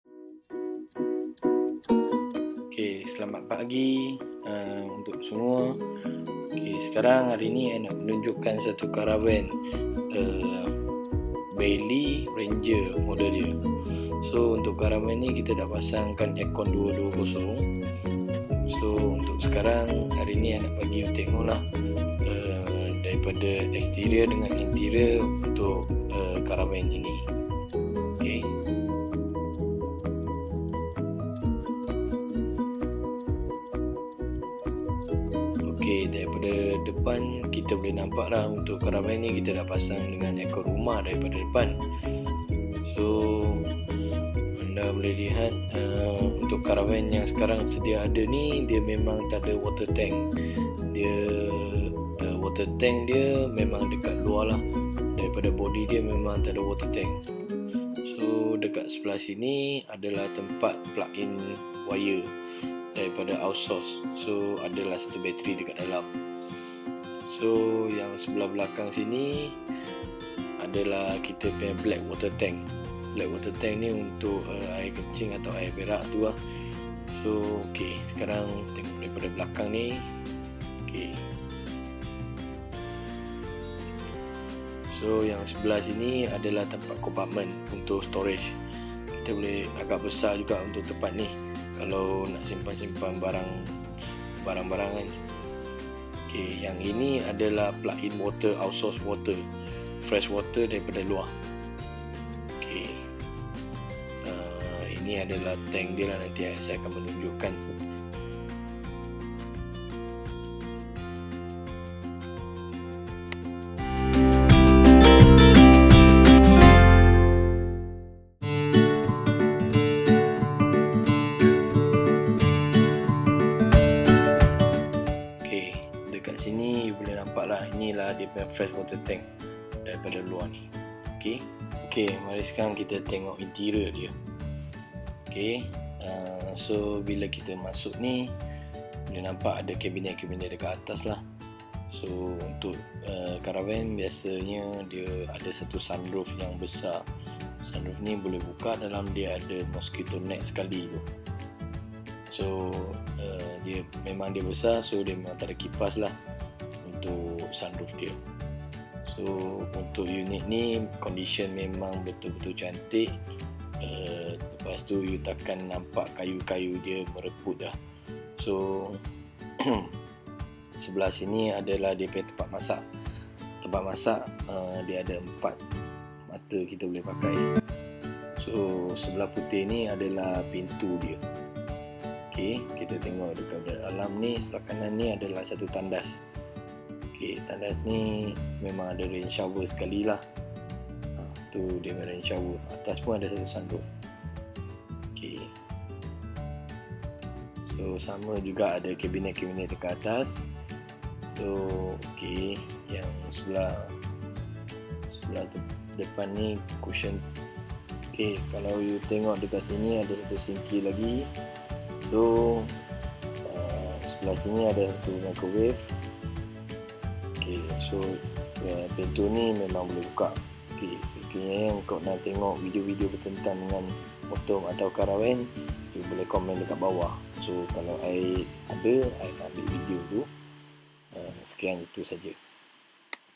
[SOLD] FOR SALE BAILEY RANGER CARAVAN (MALAYSIA )
This video is about BAILEY CARAVAN
4 BERTH
30 L FRESH WATER
45L GREY WATER TANK
220V AIRCON